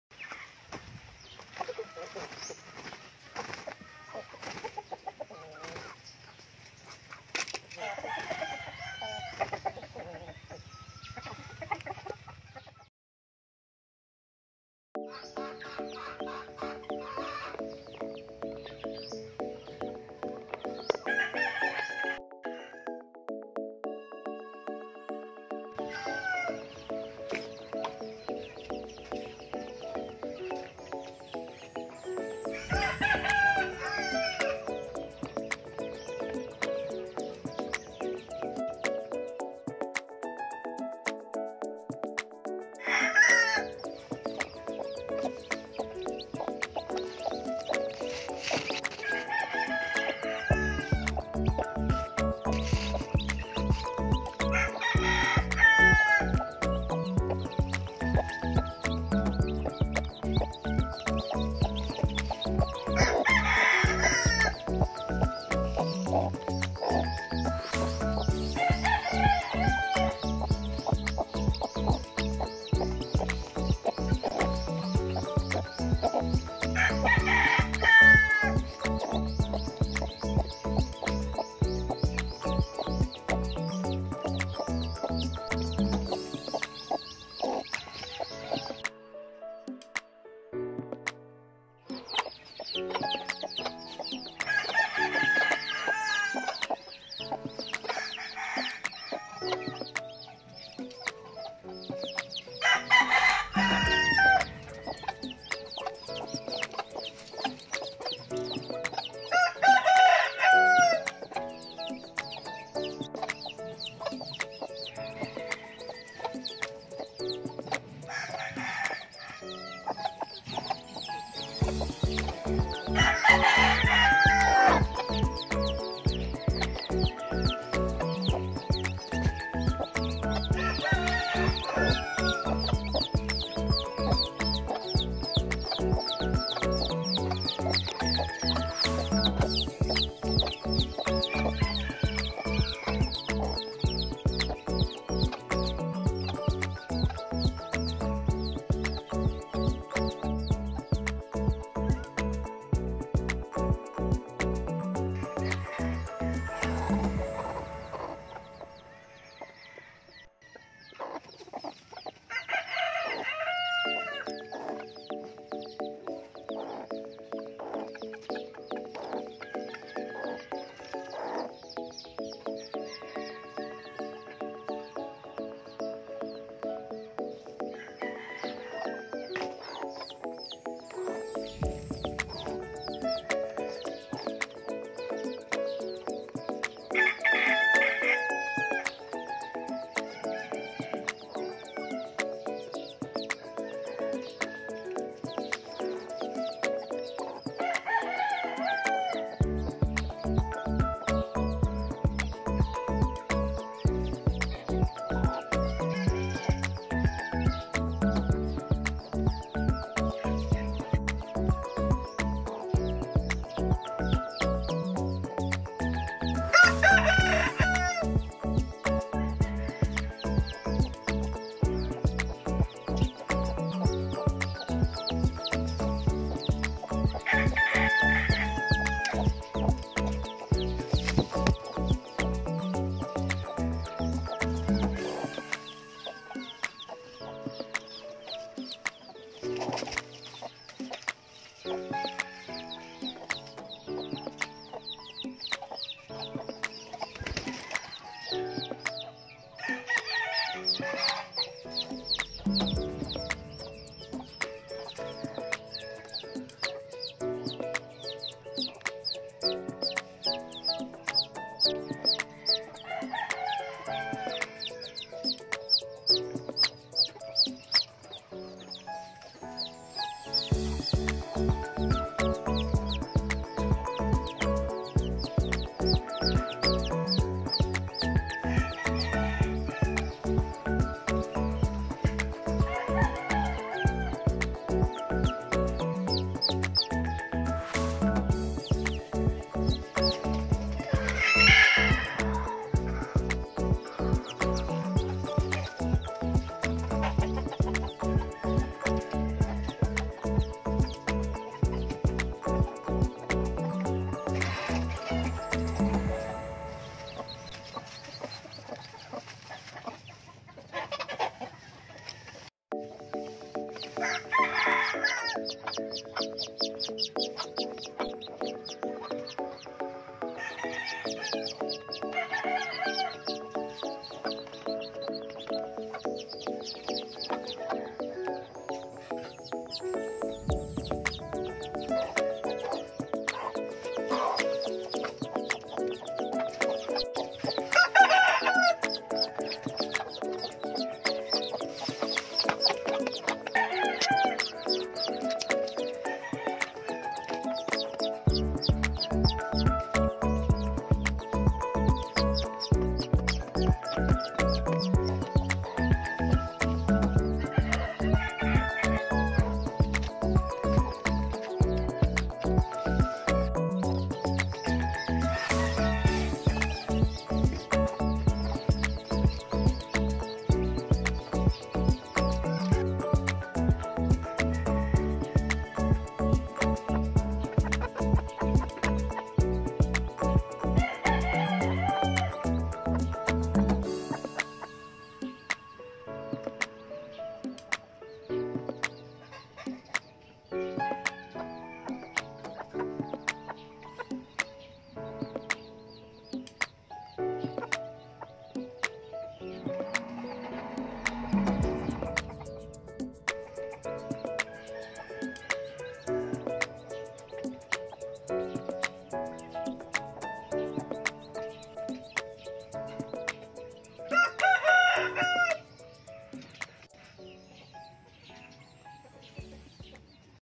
Black "JOLOANO" Hen Hatching Eggs | Natural Egg Hatching Process || Elizalde's BackYard
Black "Joloano" Hen Hatching Eggs. From mating to hatching eggs.

Let's find out:
* How are eggs hatched naturally?
* What to do after hen hatched eggs?

Natural Egg Hatching.
Natural Brooding.
Free Range Chicken Farming.

Itim na JOLOANO Native Chicken na inahing pumipisa ng mga itlog.  Mula sa pagsasama hanggang sa pagpisa ng mga sisiw.

 Alamin Natin:

* Paano magpapisa ng sisiw sa paraang natural?
 * Ano ang gagawin pagkatapos mapisa ng manok ang mga sisiw?

 Natural na Pagpisa ng Itlog.
 Natural Brooding.